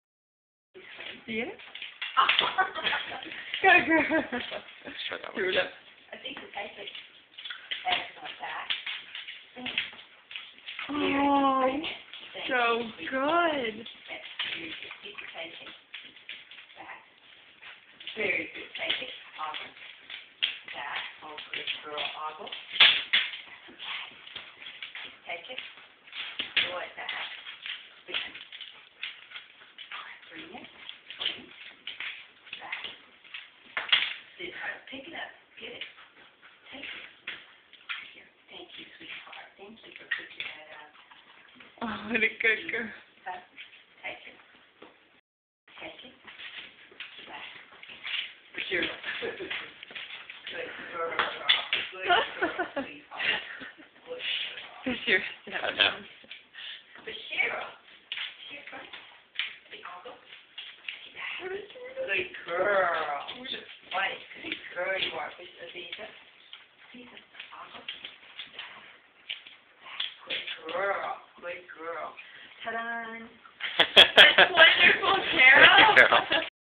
Basheera, Aziza and Suri 3
Carol Pearce's freestyle trained salukis and whippet.

Aziza = grizzle saluki
Basheera = black and tan saluki
Suri = whippet (15 mos)
Puck = lab